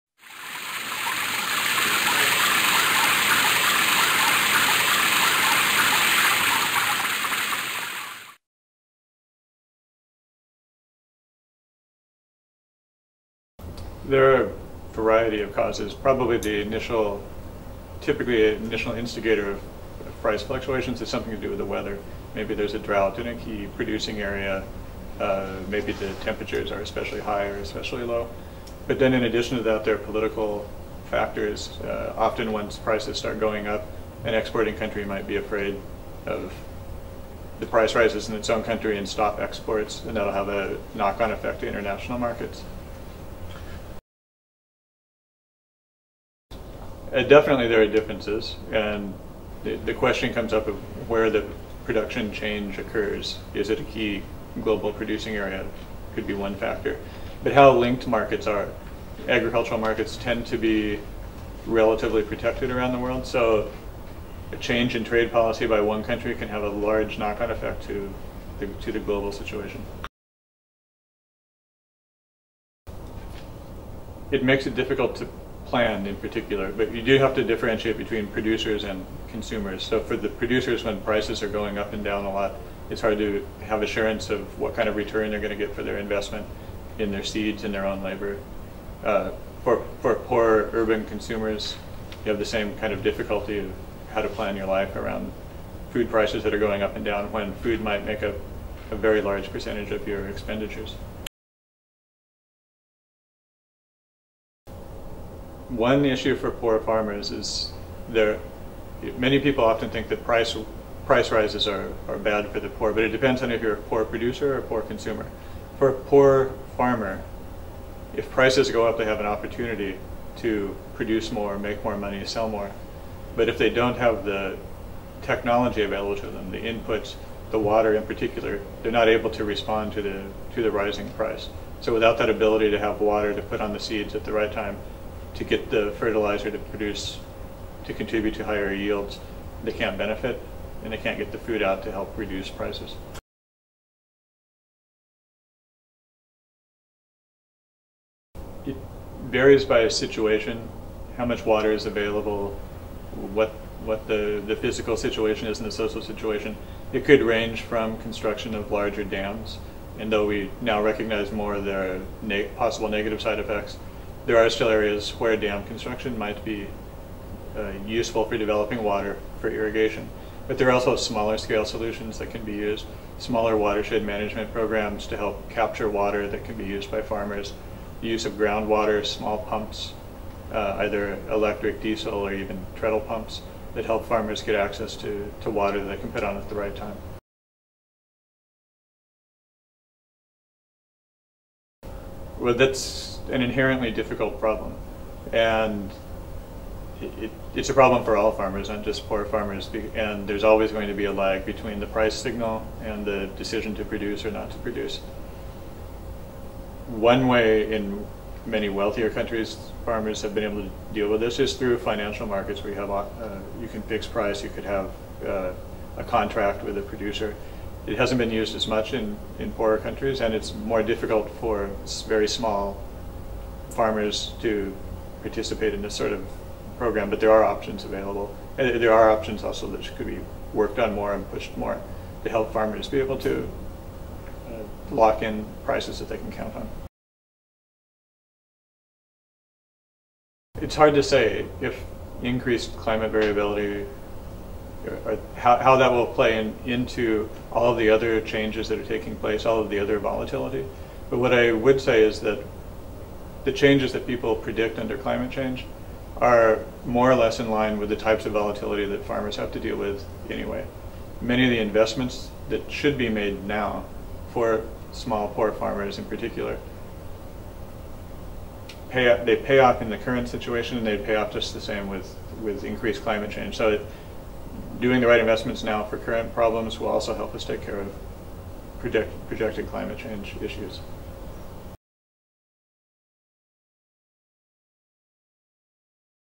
Water and Food Price Stability - Dr Mark Giordano
Food prices: from crisis to stability is the theme of this year's World Food Day, but what role does water play? Dr Mark Giordano answers some important questions in this video from the 'Think Beyond the Tap' video series from IWMI.